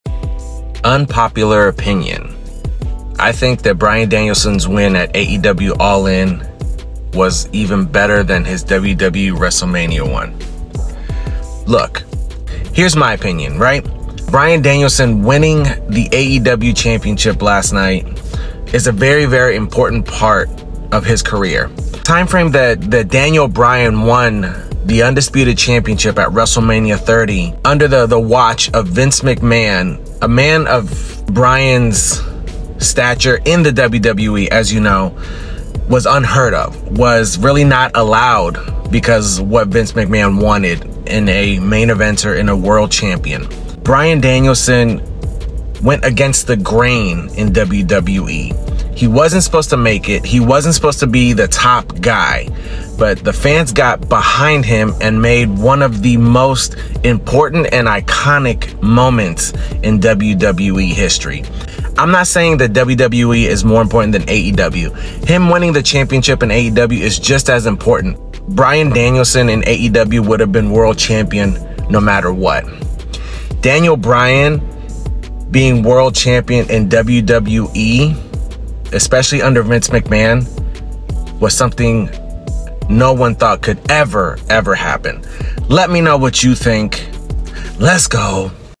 ALL IN WAS BIGGER THAN WRESTLEMANIA 30 !
In this video, we dive deep into the monumental return of Bryan Danielson at AEW All In and explore the burning question: Was AEW All In bigger than WrestleMania 30 for Bryan Danielson? We break down the significance of his career-defining moments and analyze what moment was bigger for the wrestling legend. From his historic victory at WrestleMania 30 to his epic match at All In, we compare these two unforgettable events and discuss their impact on Bryan Danielson’s legacy. Don’t miss out on this in-depth discussion of one of wrestling’s greatest stars!